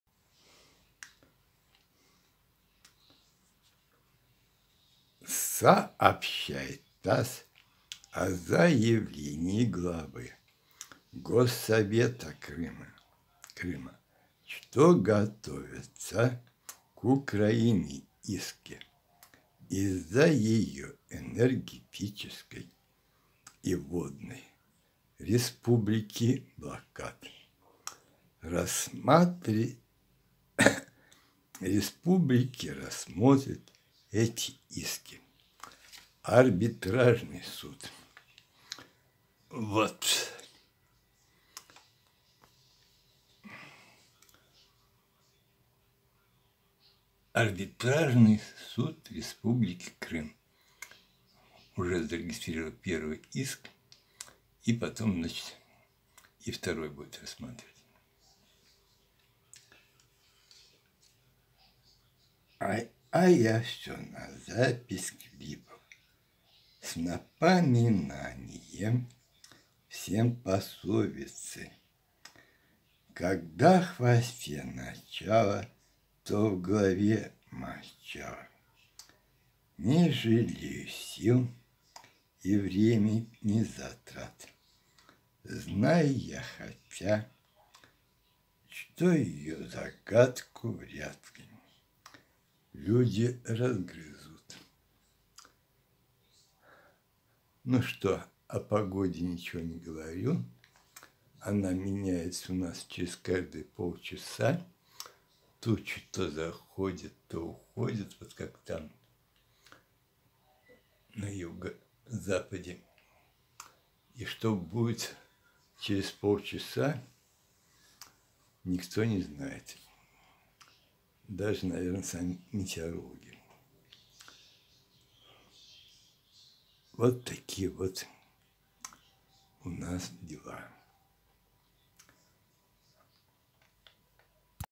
Об исках Крыма к Украине сообщает ТАСС
Сообщает ТАСС о заявлении главы Госсовета Крыма, что готовятся к Украине иски из-за ее энергетической и водной республики блокад.
Республики рассмотрит эти иски Арбитражный суд.
А я все на запись клипов с напоминанием всем пословицы – «Когда в хвосте начало, то в голове мочало» – не жалею сил и времени затрат.
Знаю я хотя, что  ее загадку вряд ли люди разгрызут.